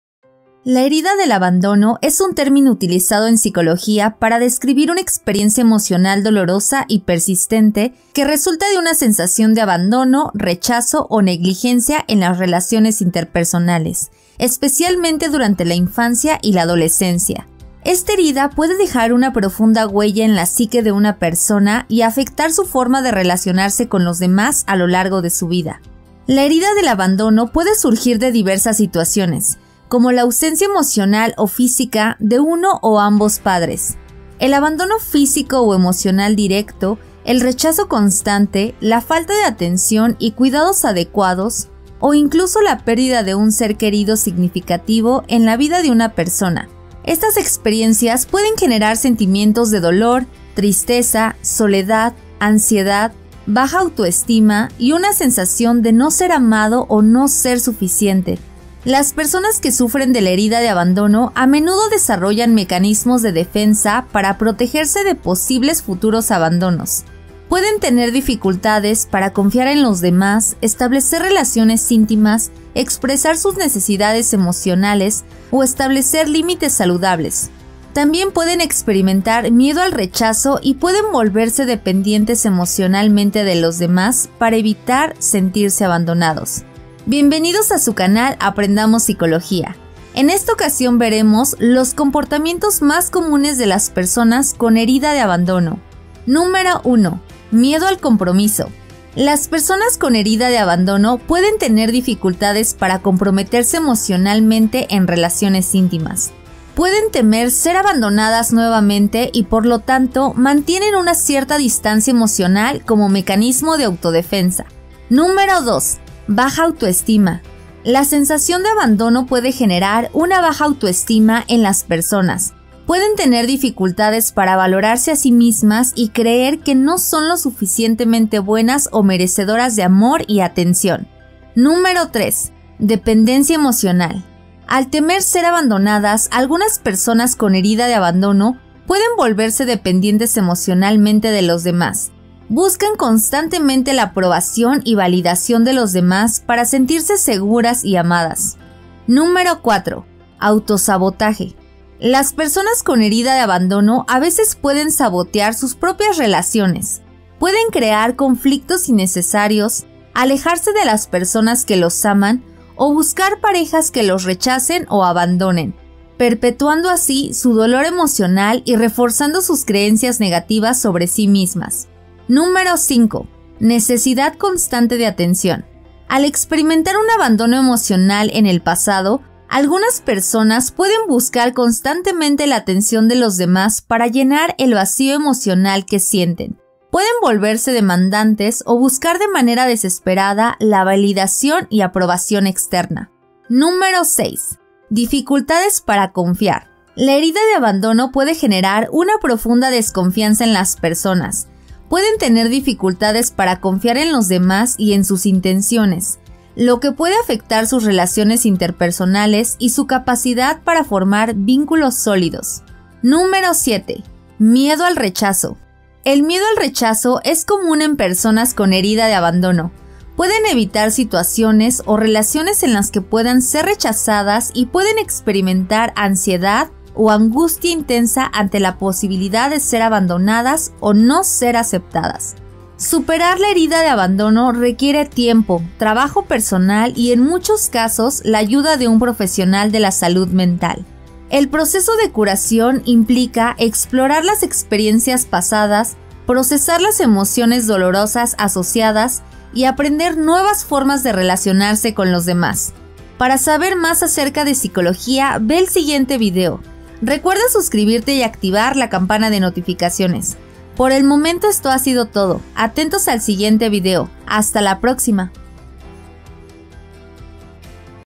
Explorando la Herida de abandono: El impacto del abandono
Las personas que han experimentado heridas emocionales de abandono a menudo exhiben comportamientos característicos en sus relaciones y vidas diarias. Estas heridas pueden surgir de diversas situaciones, como la pérdida de un ser querido, una separación traumática o experiencias de negligencia emocional durante la infancia.
Las personas con herida de abandono pueden mostrar una serie de comportamientos, como el miedo intenso a la intimidad, la dificultad para confiar en los demás, la tendencia a sabotear sus propias relaciones o aislarse socialmente. También pueden experimentar una baja autoestima, sentirse constantemente inseguros o tener miedo de ser abandonados nuevamente.
Estos comportamientos pueden surgir como mecanismos de defensa para protegerse de posibles heridas emocionales adicionales.